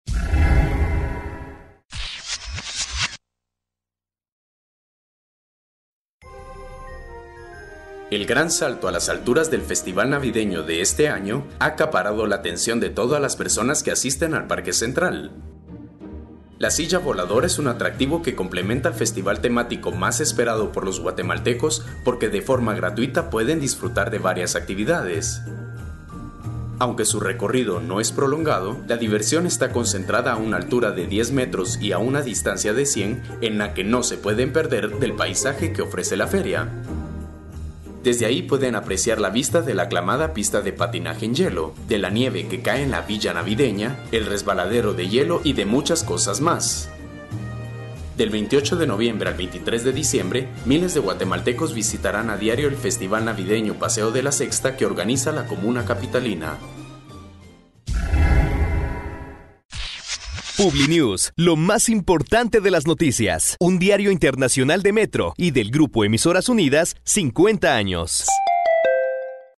Un viaje navideño de altura en la silla aérea
La silla voladora es un atractivo que complementa al festival temático más esperado por los guatemaltecos porque de forma gratuita pueden disfrutar de varias actividades

Por: Luis Carlos Nájera / Oliver De Ros